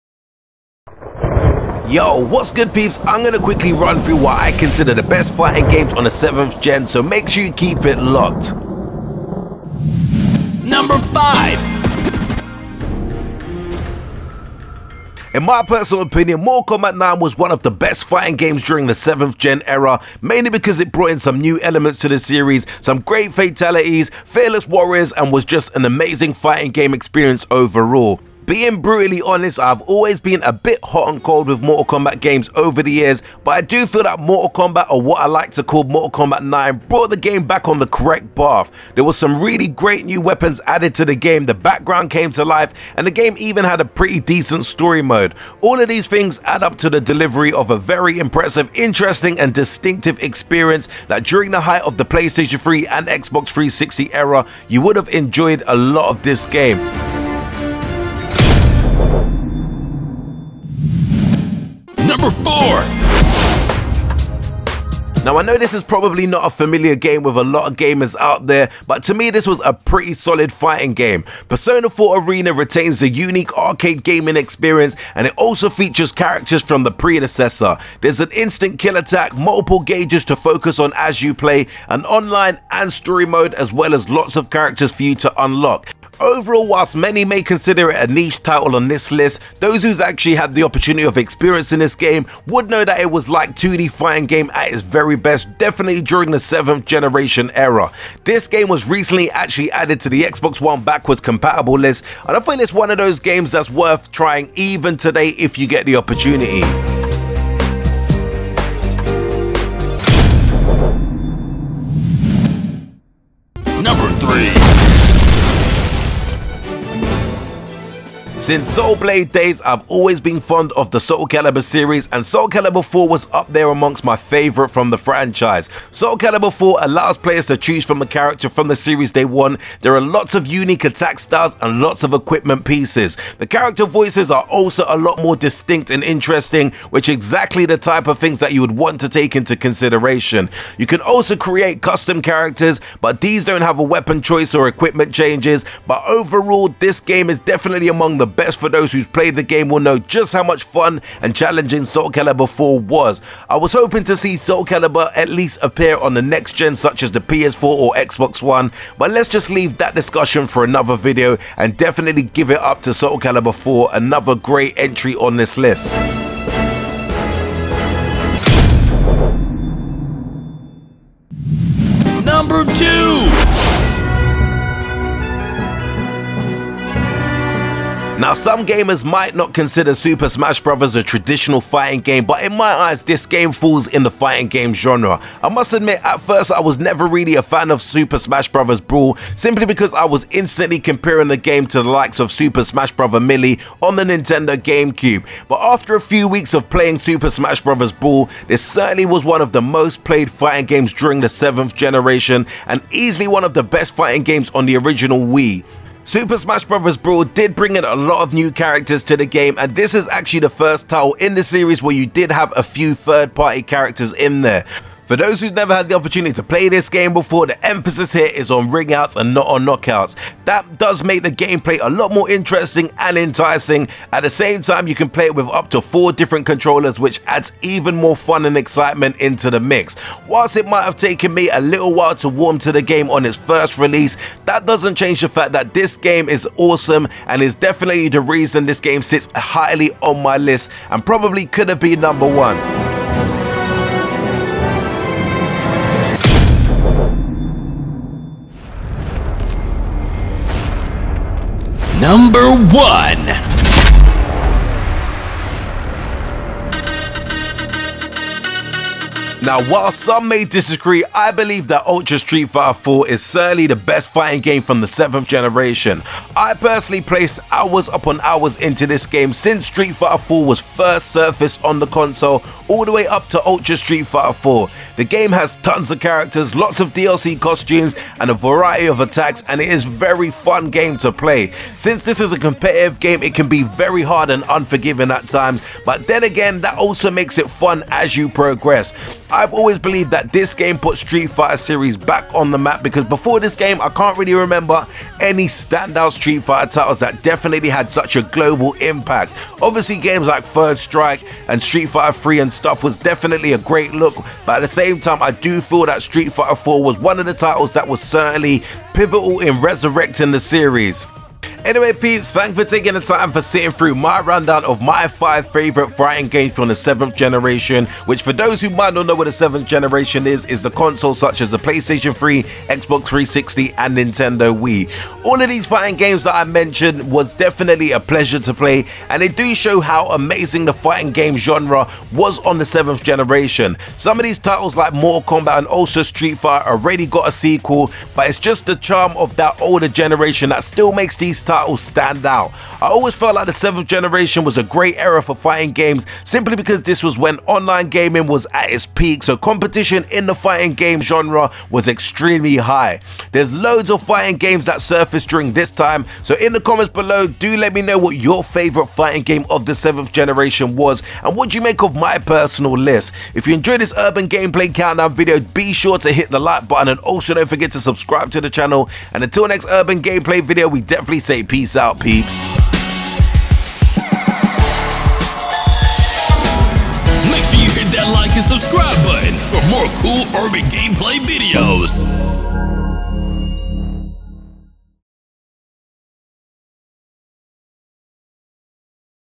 Best Fighting Games Xbox 360, PS3, Nintendo Wii
Fighting games on the 7th generation of consoles were outstanding, not only because they got to innovate a lot, but they also offered a smooth and fun gameplay. The best part about these is that they work great on these consoles and each game comes with its own unique variety of actions and so on!